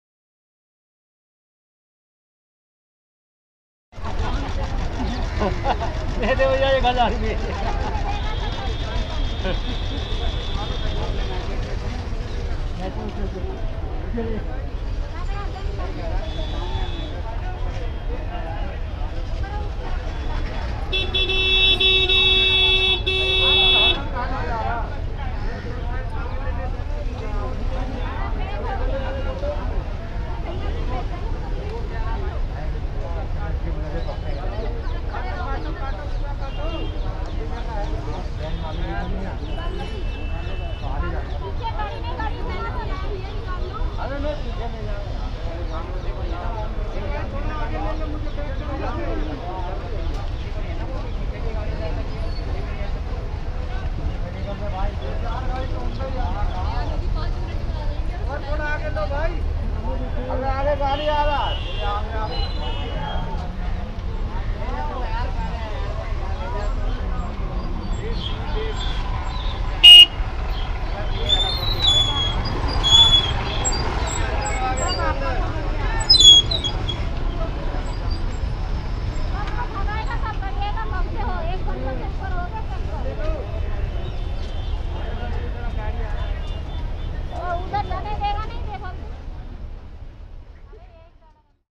मोरी गेट का ये रोज़ का हाल है। दिल्ली में जाम | सिर्फ़ बस फ्री करने से कुछ नहीं होता
Please share to show the reality to the public